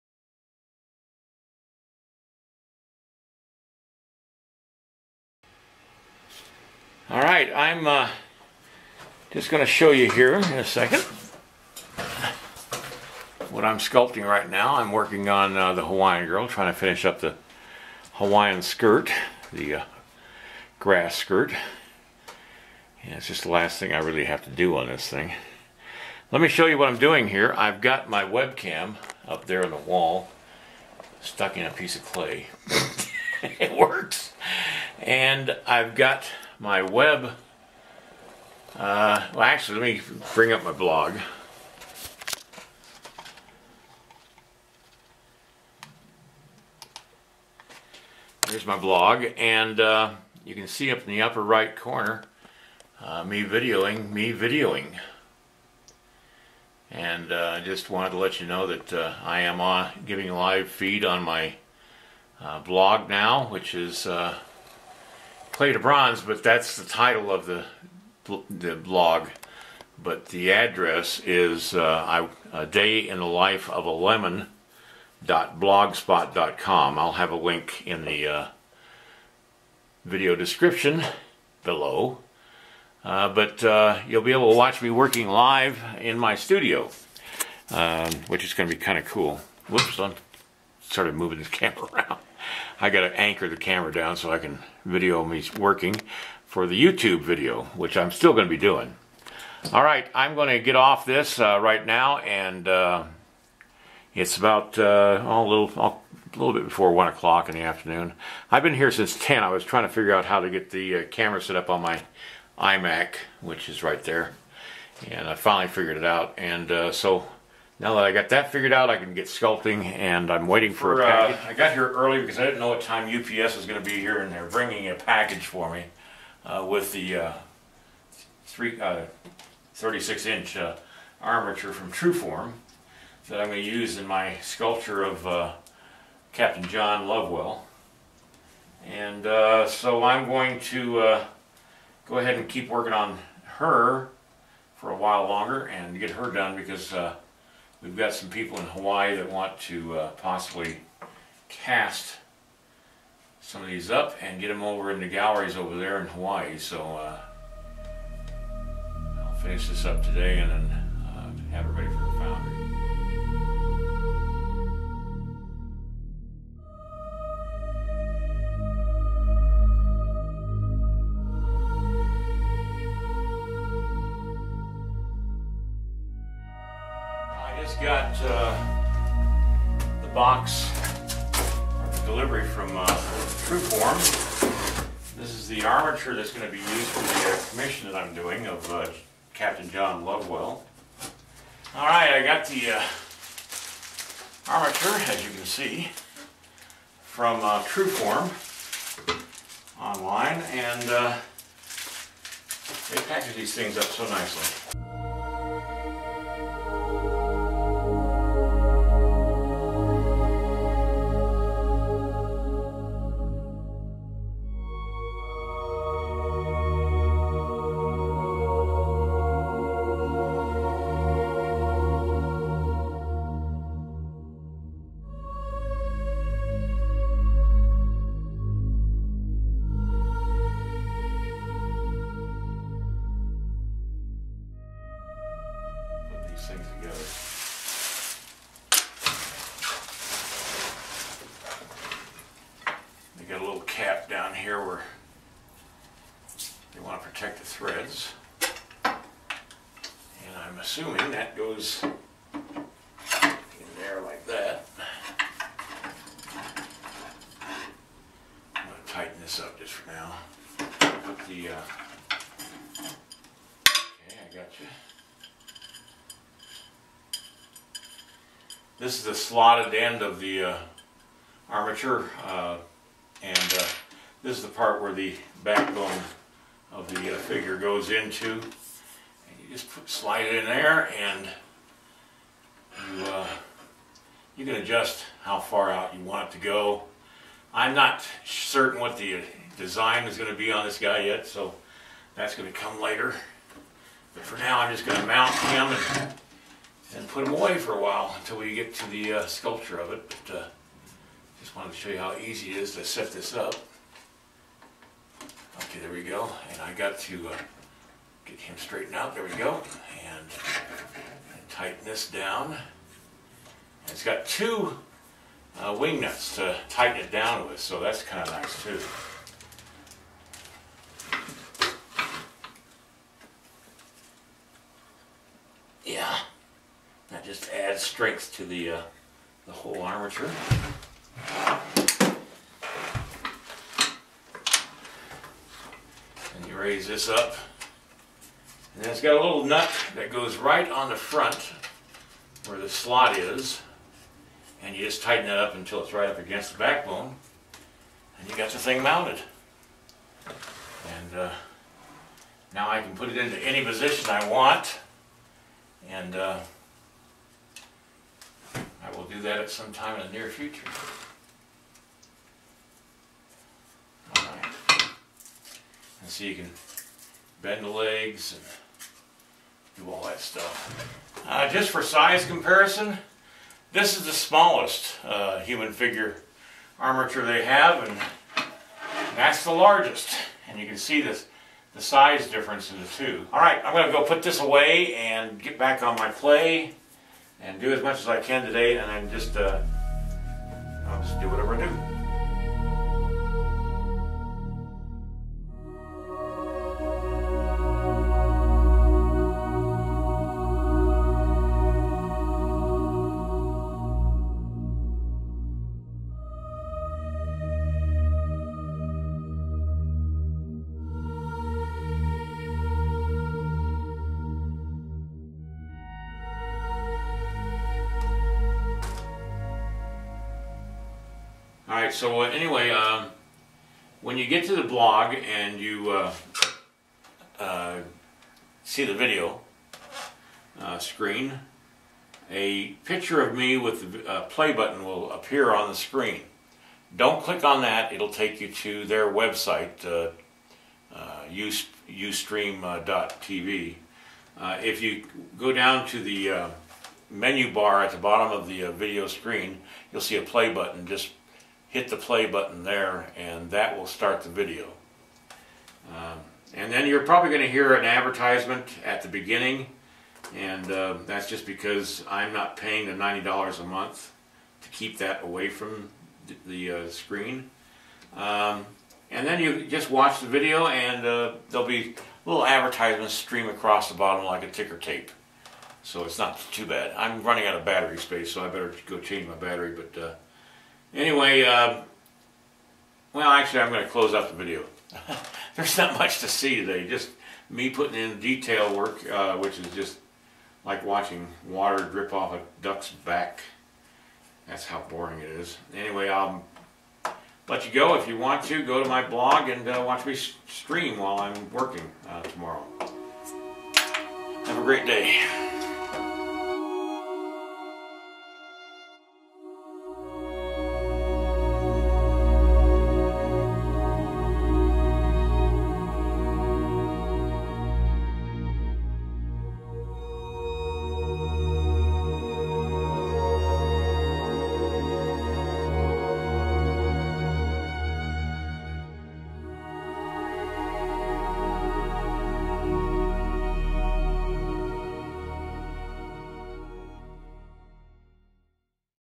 My 36 inch Human TruForm Armature Arrives - First Day of Live Studio Video Streaming
1st day of going studio live on the web. I was a bit disjointed in getting going today. Did manage to finish the Hula Dancer, and received the Truform armature for the commission I'll be starting next week.
If you want to watch the live feed, go to:
between the hours of 1 pm and 4 pm mountain time weekdays.